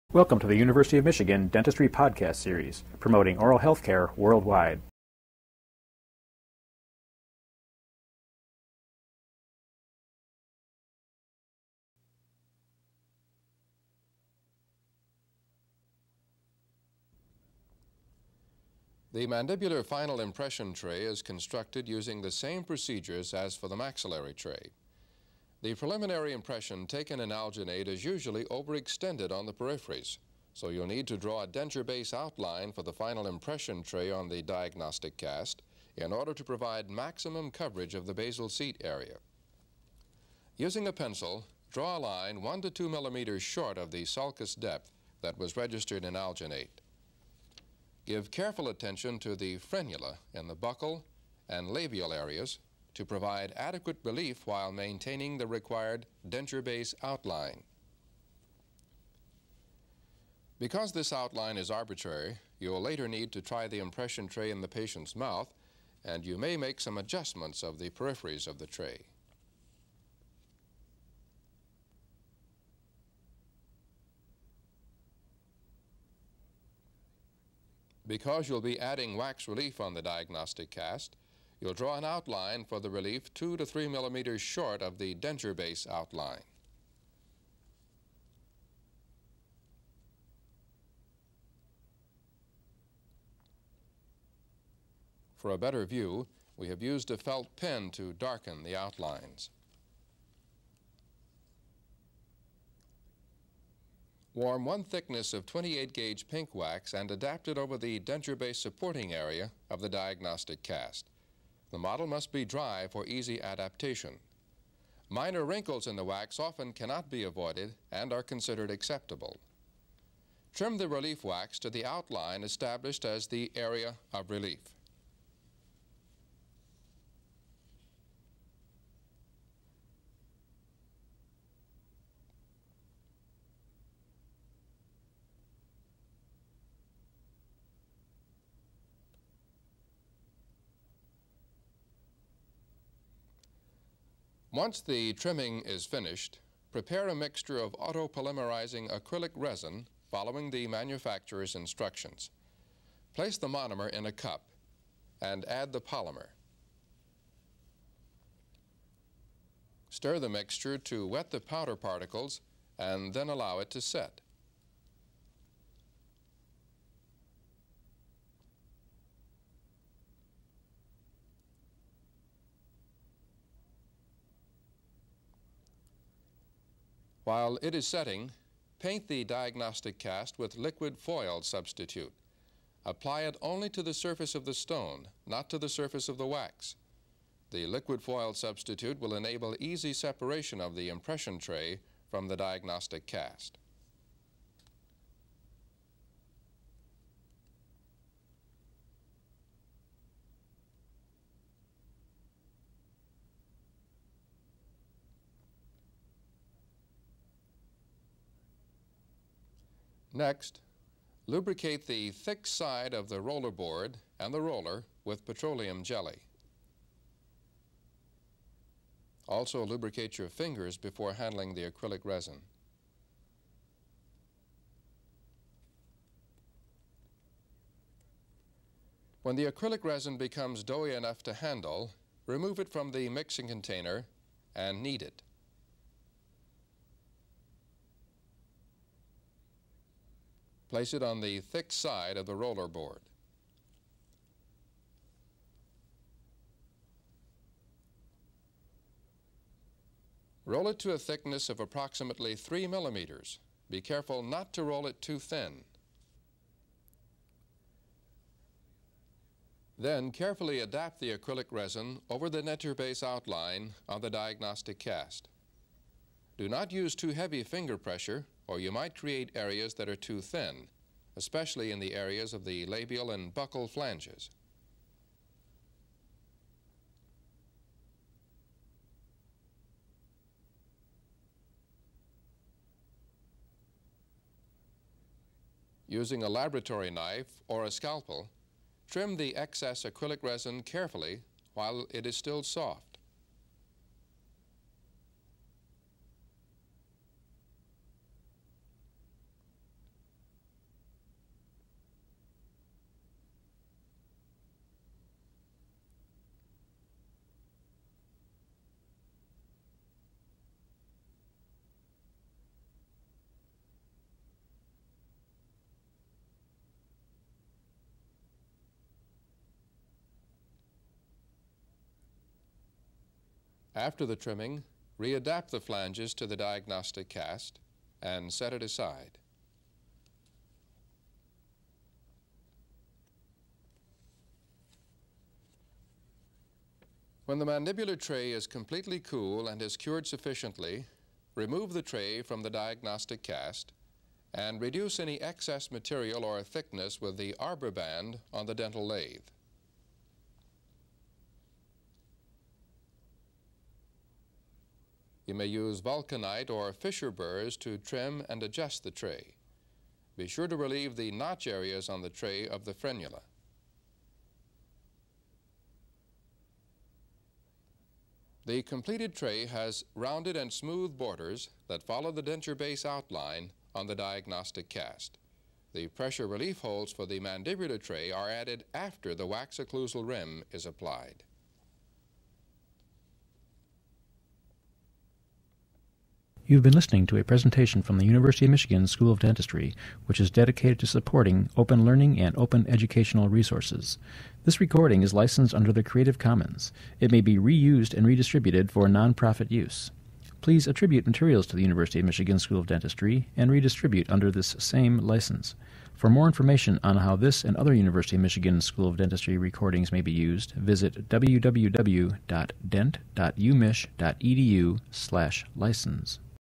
Custom Acrylic Resin Tray Construction: Mandibular Final Impression
Procedure for fabrication of a custom acrylic resin tray for taking the mandibular final impression.  Orig. air date: JUN 21 77